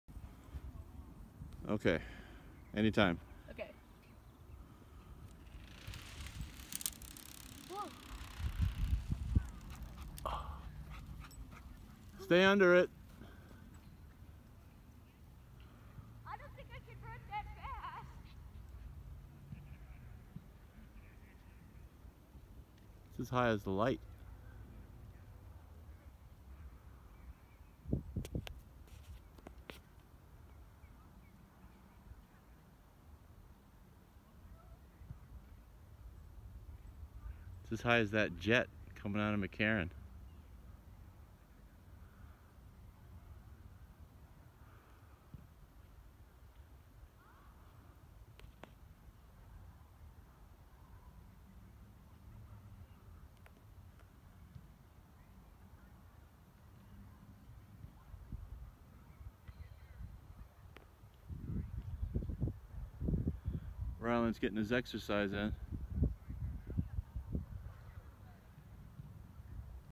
Flying an AMA cub at Henderson park.
Flying a rubber powered AMA cub at the park.